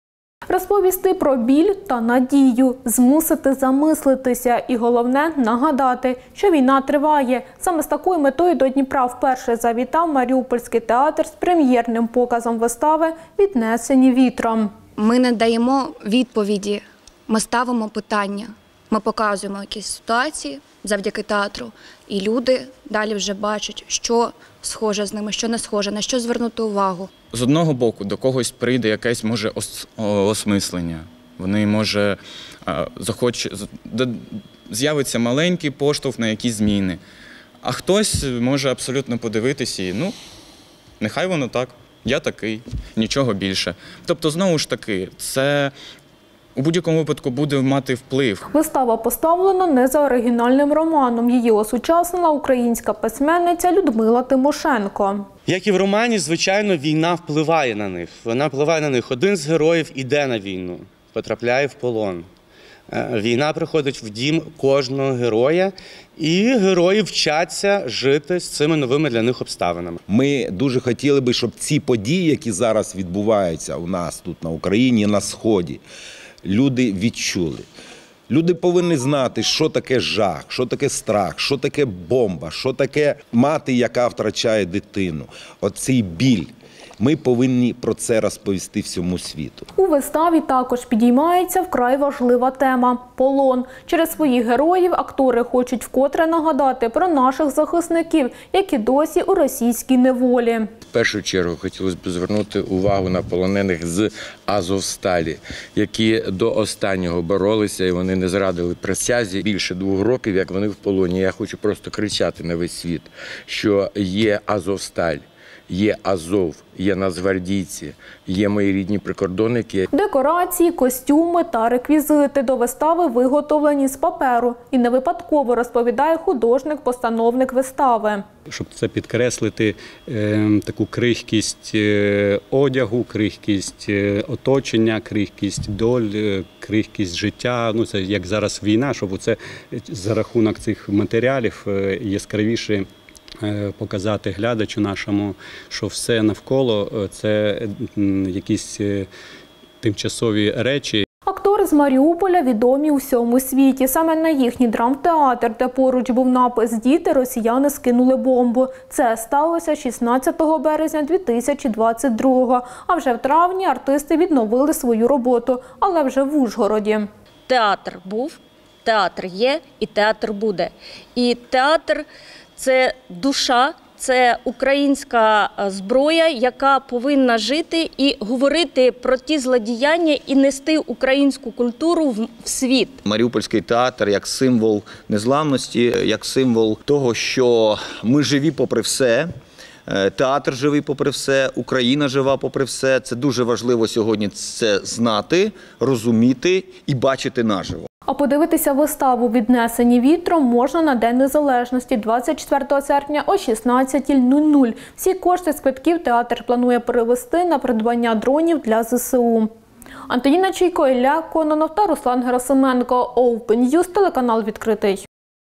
Маріупольський театр завітав у Дніпро з прем’єрою
Відроджені з пекла. У Дніпро завітав Маріупольський театр із прем’єрою під назвою «Віднесені вітром». Але твір осучаснили. У виставі - події, які нині відбуваються в нашій країні. Чого очікувати глядачам? Та коли актори вийдуть на сцену?